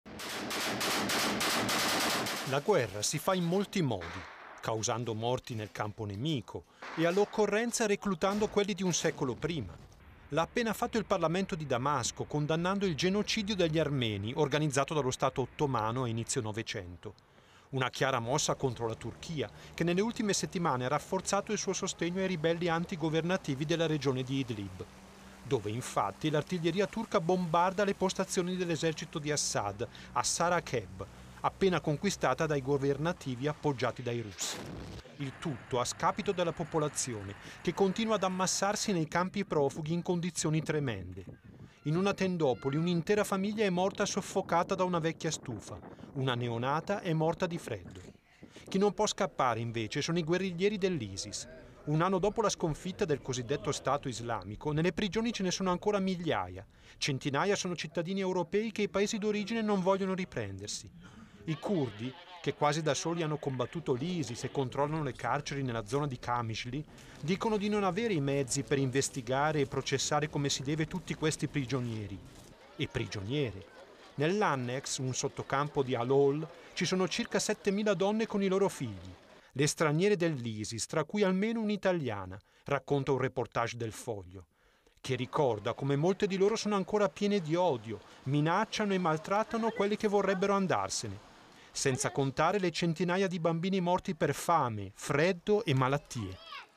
Siria, la Turchia spara sulle truppe di Assad
In Siria continuano i combattimenti intorno a Idlib e la fuga dei civili. Il servizio di Marco Burini